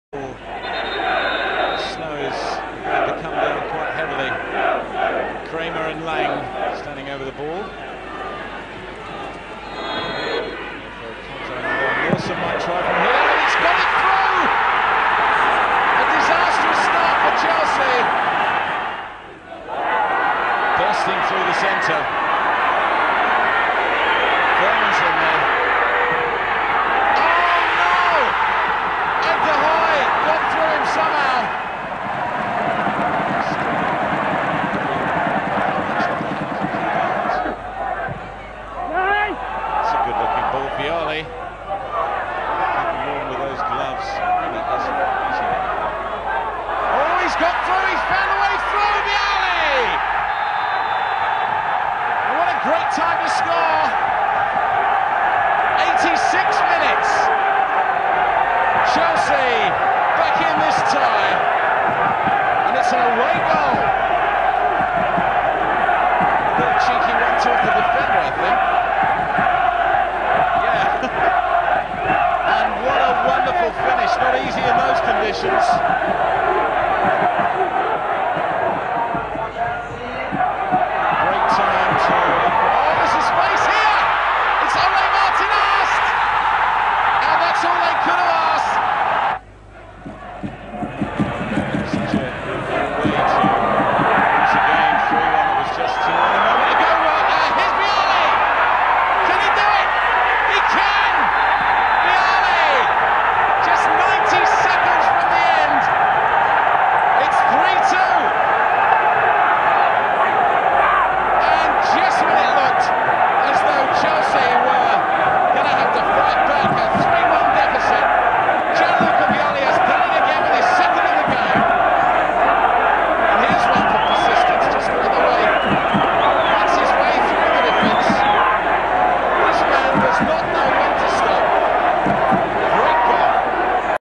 Tromsø 3-2 Chelsea {23.10.1997}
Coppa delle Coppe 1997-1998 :
Ottavi di finale andata

Goals :
6' S.Nilsen (Tromsø)
19' Fermann (Tromsø)
85' G.Vialli (Chelsea)
86' Arst (Tromsø)
90' G.Vialli (Chelsea)